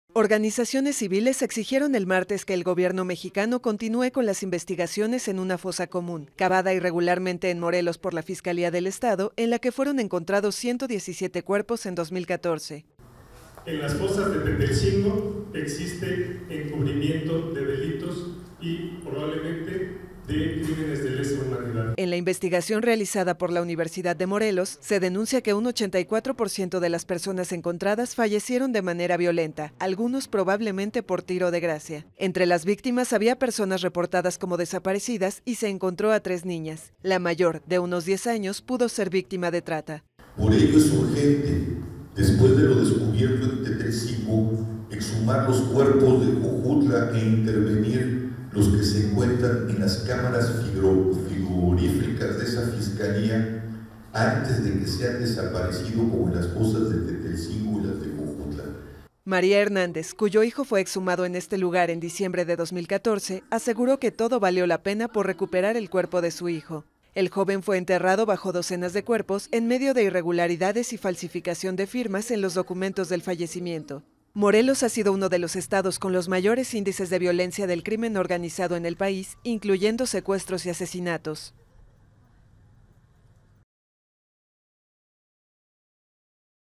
Activistas denuncian encubrimiento de delitos en fosas en México
Organizaciones civiles y familiares de personas desaparecidas en México, denunciaron el martes que en las fosas irregulares halladas en el estado de Morelos, y cavadas por las autoridades, existe encubrimiento de delitos y exigieron al gobierno continuar las investigaciones.